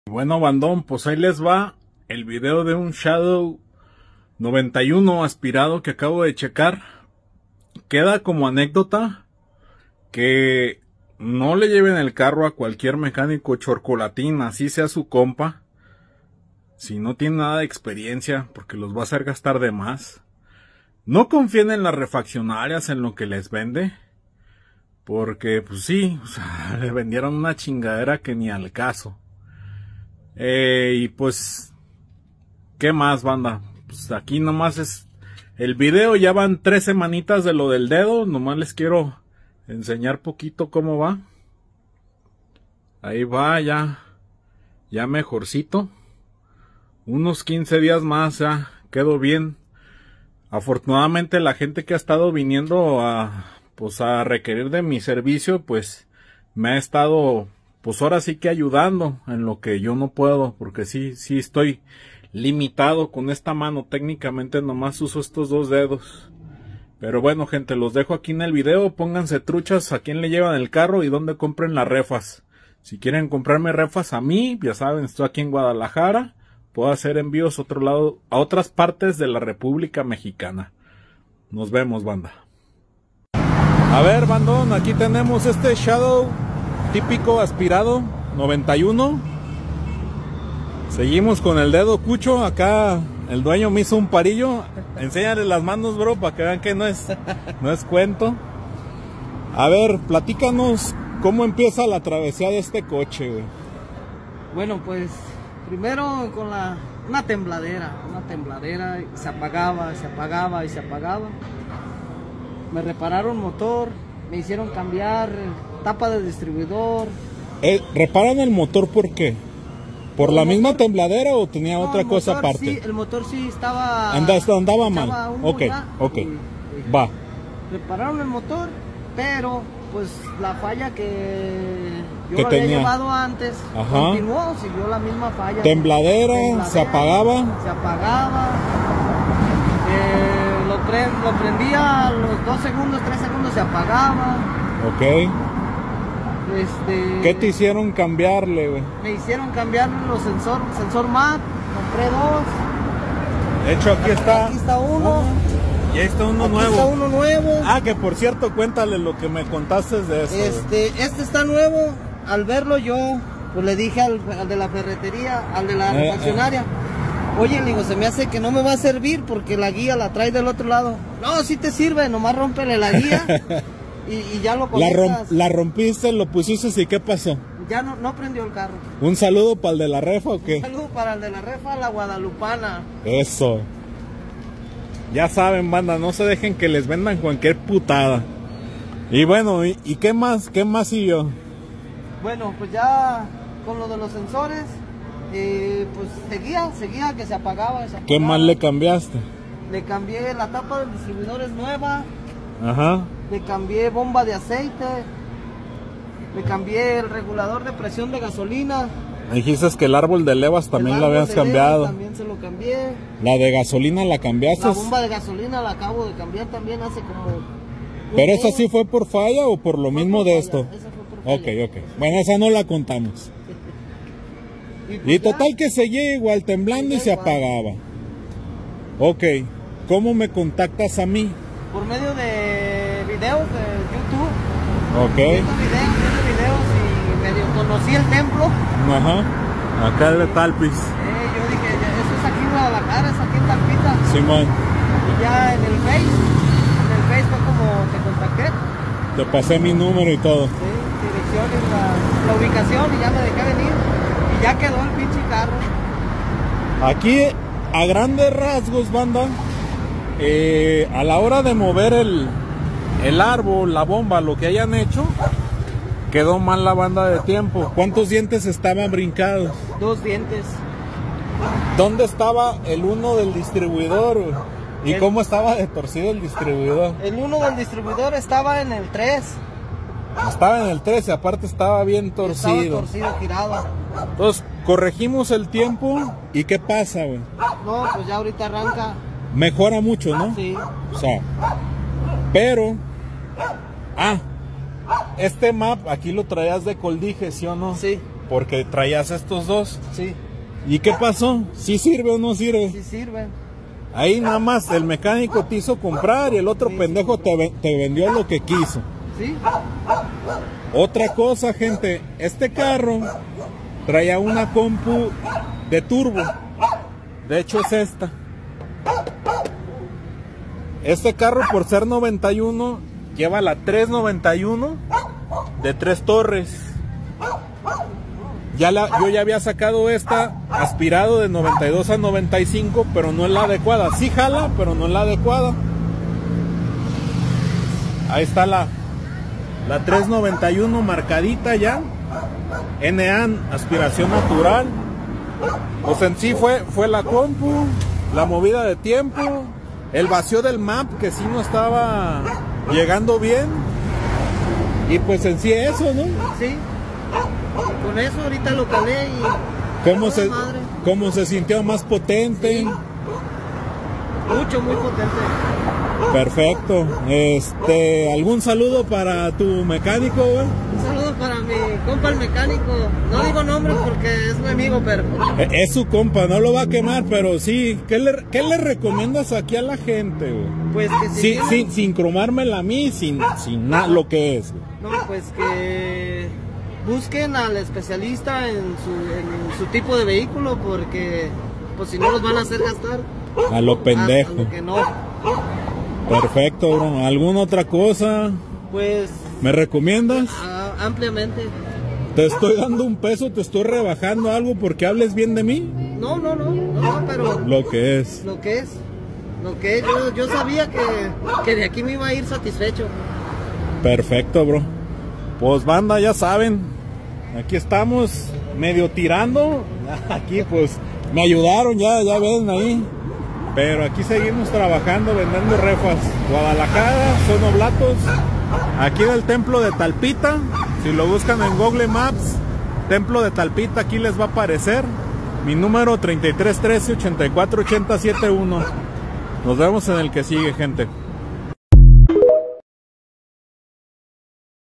shadow 91 aspirado con multifallas y multimmds.
bueno gente... aqui tenemos otro tipico caso de cambiale cosas haber si con eso queda!.. aunado a refacciones que no sirven o no debe de llevar (como la compu de turbo) cuando se encuentren a un mecanico asi o una refaccionaria chuky.. corran de hay y cuentenle a quien mas confianza le tengan 🤣🤣🤣
seguimos trabajando!